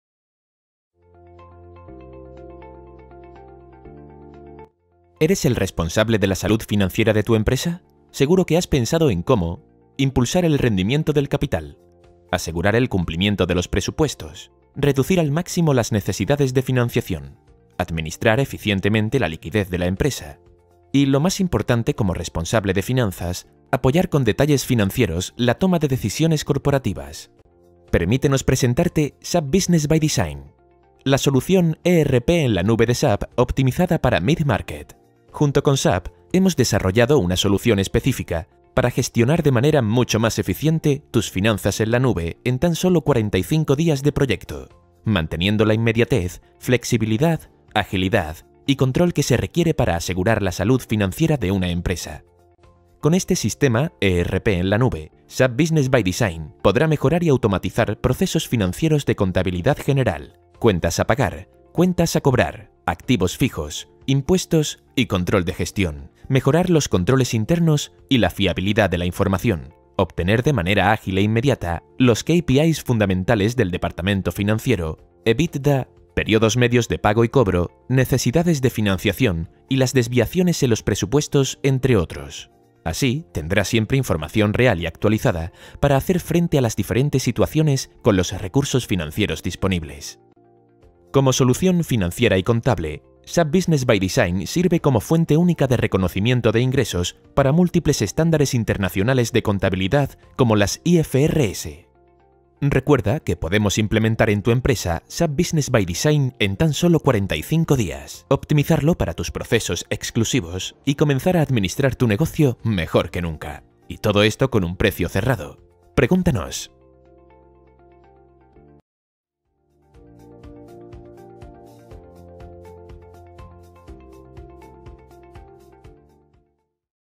SAP FI 45 días - Software gestión financiera para mid market
Impulsa las Finanzas de tu compañía con el ERP Cloud: SAP Business ByDesign.

Te contamos qué es este software de gestión financiera para empresas de tamaño medio, qué tienen grandes previsiones de crecimiento, cómo sacarle el máximo beneficio, y porque es uno de los mejores del mercado.

Si eres Director Financiero y diariamente te enfrentas a retos como:
- Trabajar con un dato único.
- Cierres contables fluidos y rápidos.
- Ganar Transparencia o Visibilidad de estados en tiempo real.

SAP FINANZAS EN 45 DÍAS, es la solución perfecta para ti, certificada por SAP, y que podrás implementar en tan solo 8 semanas de proyecto y con un precio cerrado.